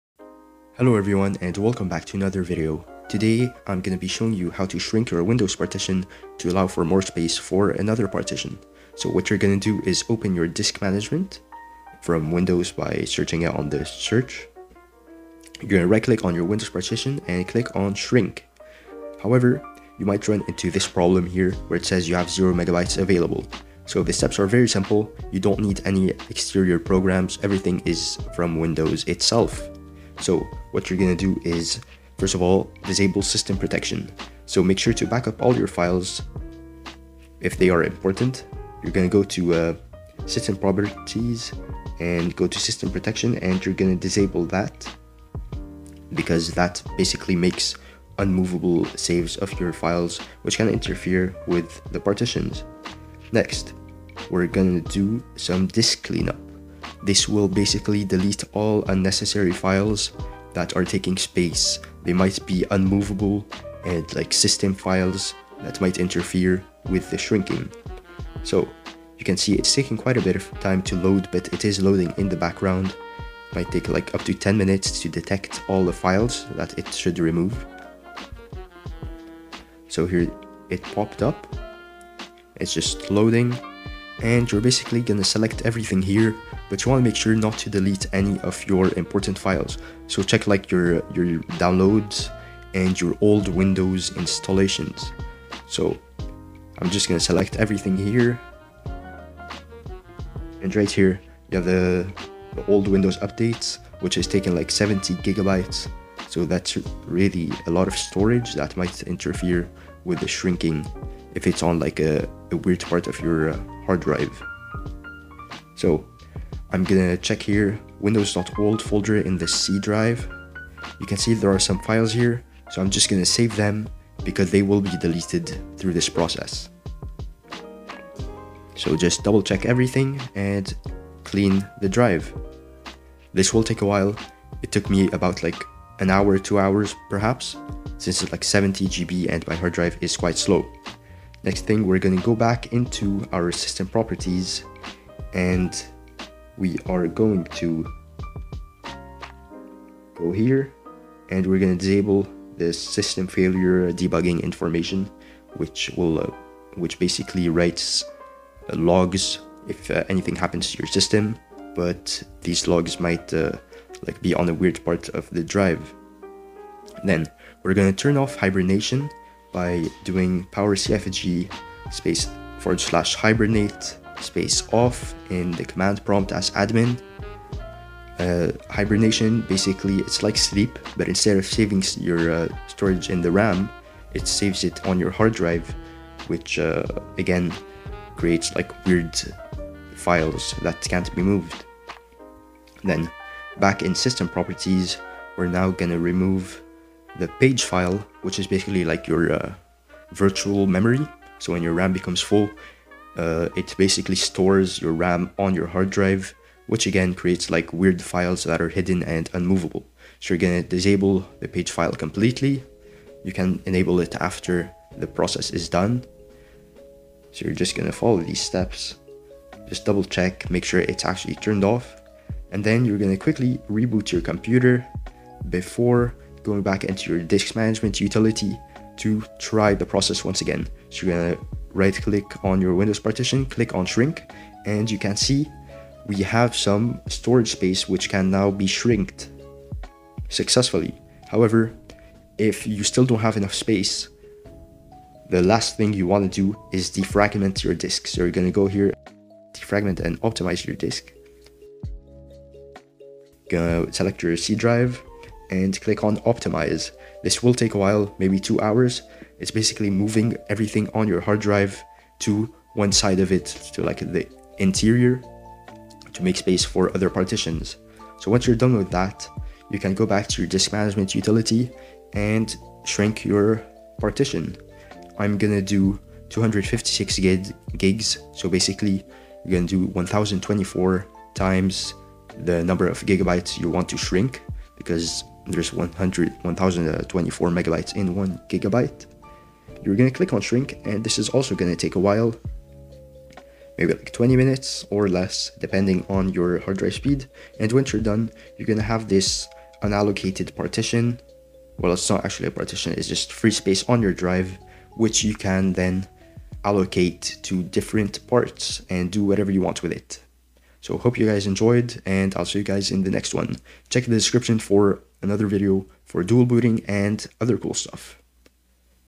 How to shrink Windows Partition | Fix 0 MB Problem (no external tools)
~ How to shrink your windows partition 0MB

Hello everyone, welcome back to another video! In today's tutorial, I'll be guiding you through the process of shrinking your Windows partition to free up space for another partition. No need for external programs – everything can be done using Windows tools.

Steps:

0:00 Open Disk Management by searching in the Windows search bar.
0:17 Right-click on your Windows partition and select "Shrink."
If you encounter the issue of having zero megabytes available, follow these simple steps to resolve it.

Additional Steps:

0:30 Disable system protection to allow for movable files.
0:53 Perform disk cleanup to delete unnecessary and unmovable files.
2:20 Disable system failure debugging information.
2:46 Turn off hibernation using the command prompt.
3:12 Remove the page file (virtual memory).
3:40 Reboot your computer.

Final Shrink Process:

Right-click on your Windows partition again.
3:45 Click on "Shrink" to successfully create unallocated space.
4:00 If needed, defragment your disk to optimize space.
4:25 Shrink the partition based on your desired size (remember 1,024 megabytes = 1 gigabyte).
Allocate the unallocated space for different partitions.

Please note that these steps may take some time, so be patient. In the video description, you'll soon find a link to another video covering dual-booting and other exciting topics.